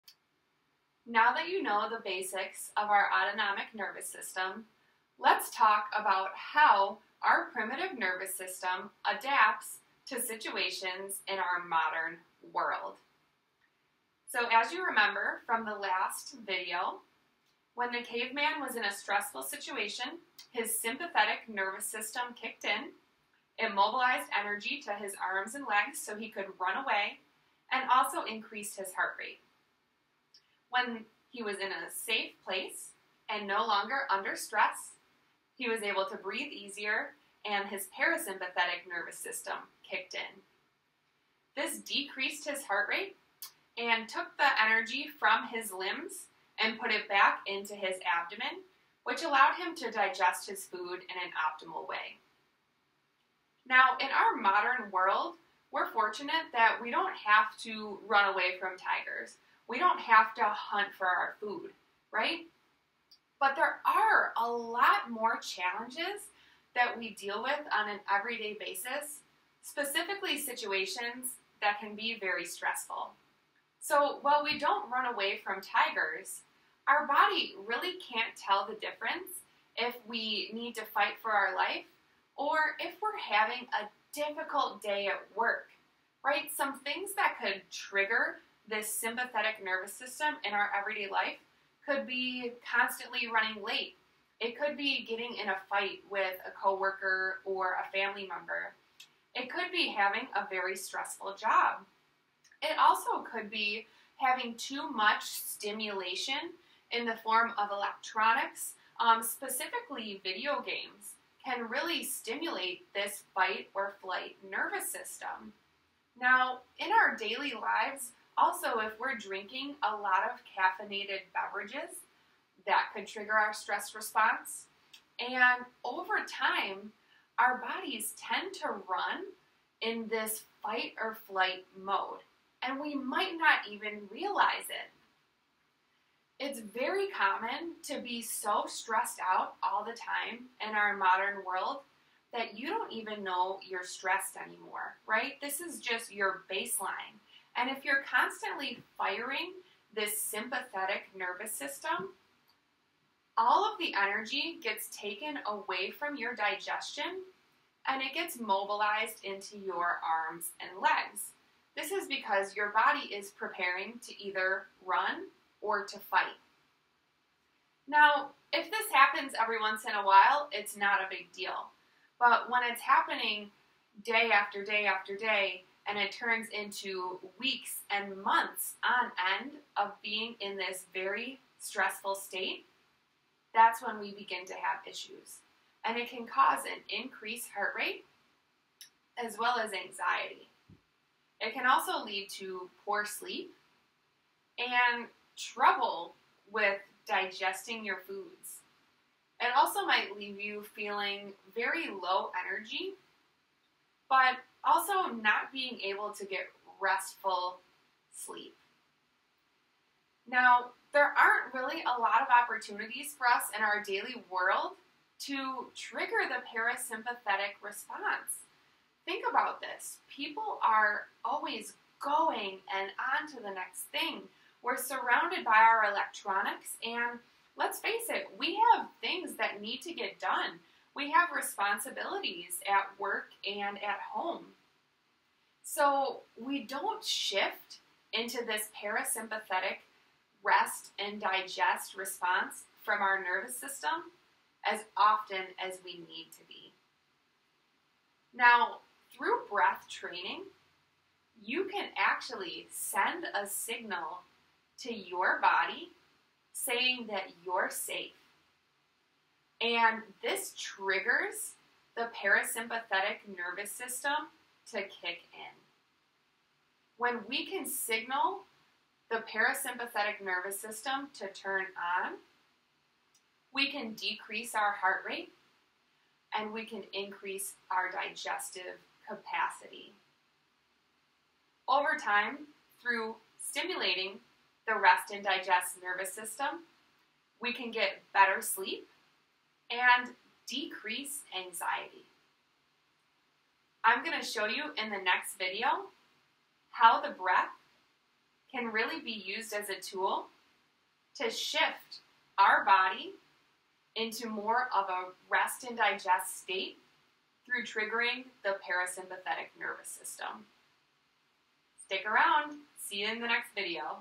Our Primitive Nervous System In A Modern World
Learn how the stressors of our modern world impact the balance between our sympathetic and parasympathetic nervous system in a negative way.  We can use breath training practices to get our nervous system back in balance!